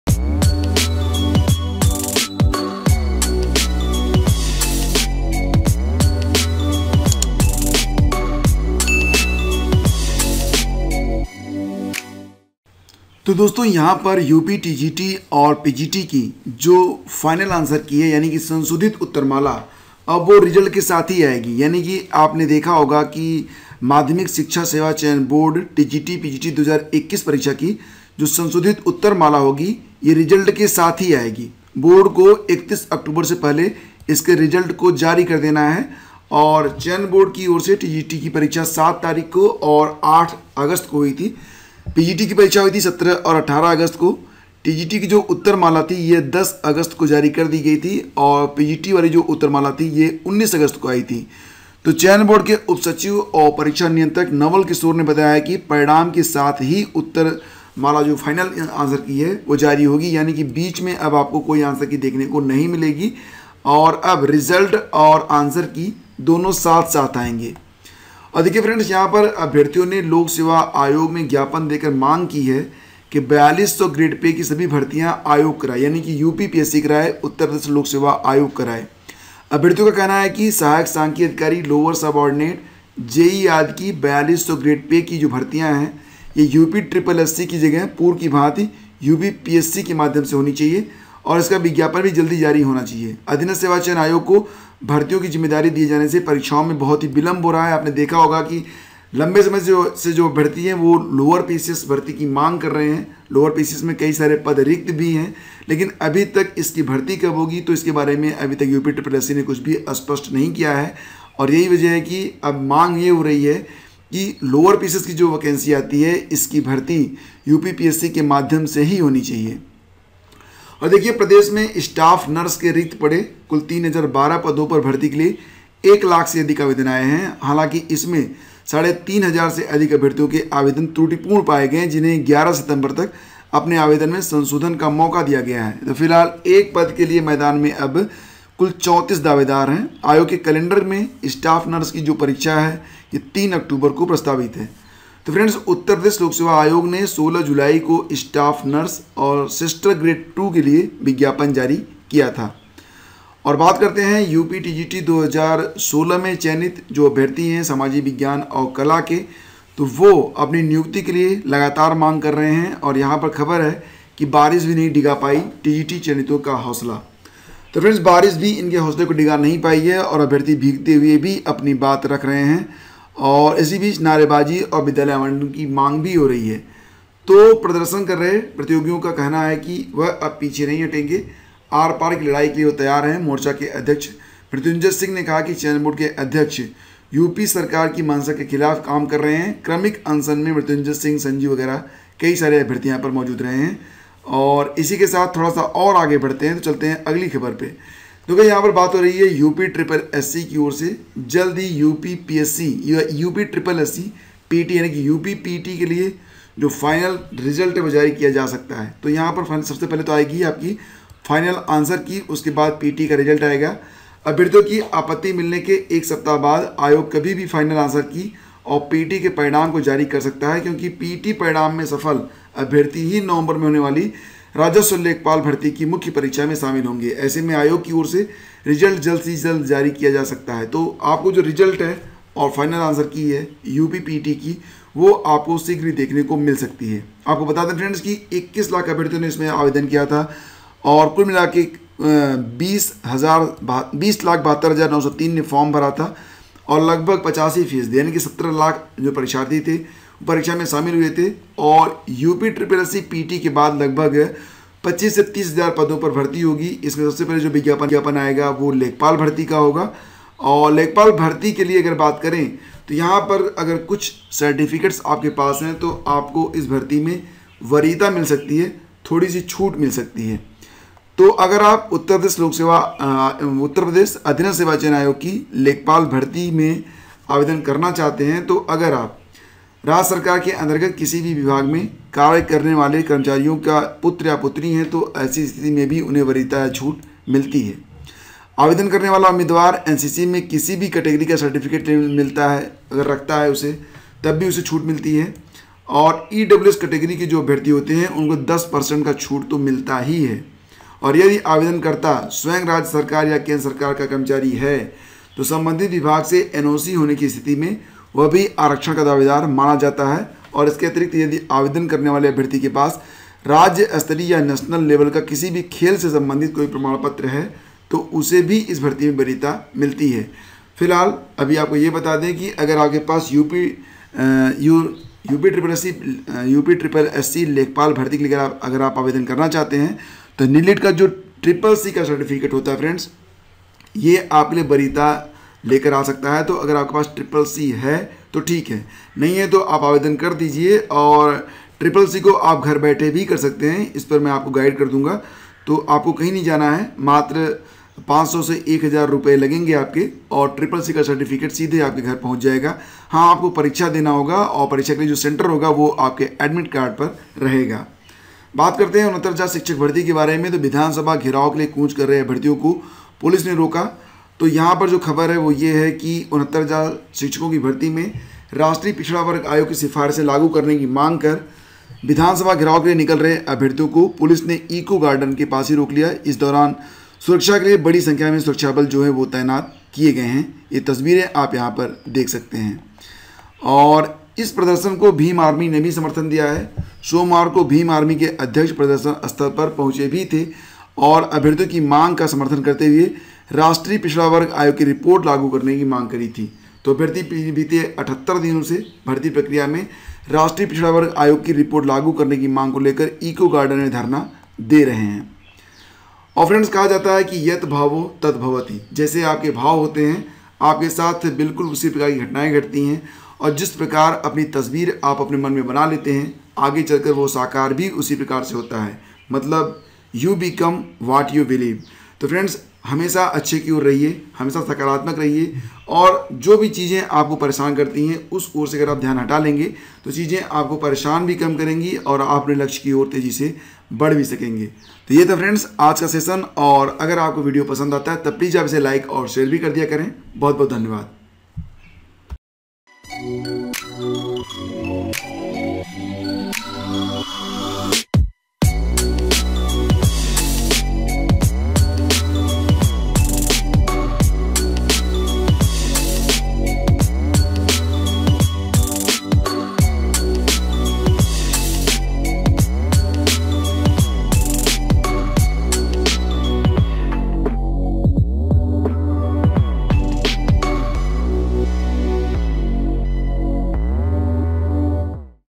फाइनल आंसर की और रिजल्ट एक साथ, लेखपाल भर्ती, रोजगार समाचार 7 सितंबर 2021,
फाइनल आंसर की और रिजल्ट अब साथ ही जारी होंगे
यूपी टीजीटी पीजीटी 2021 की भर्ती 31 अक्टूबर तक पूरी होगी
UPSSSC PET- फाइनल आंसर की व रिजल्ट
लेखपाल भर्ती में मिलेगी इन सर्टिफिकेट पर छूट या वरीयता
लोअर पीसीएस फिर से UPPSC द्वारा करानें की मांग
69000 शिक्षक भर्ती- NBC की रिपोर्ट लागू करानें को प्रदर्शन
बारिश भी नही डिगा पाई टीजीटी चयनितों का हौसला
स्टाफ नर्स के लिए संशोधन तिथि की लास्ट डेट निकट
अन्य खबरे व सूचनाए

Monthly Current Affairs PDF/ 8 Month Combined Current Affairs PDF

Study with Gyan Prakash